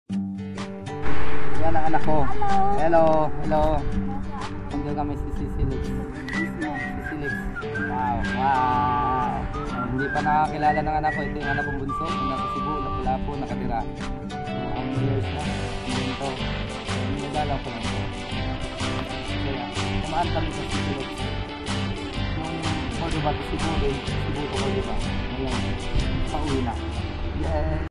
spending time with my daughter, @CCLEX, Cebu Cordova Link Expressway@ubqmotovlog6381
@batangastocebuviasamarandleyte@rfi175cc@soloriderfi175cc

SOLO ROAD TRIP from BATANGAS TO CEBU , Via SAMAR and LEYTE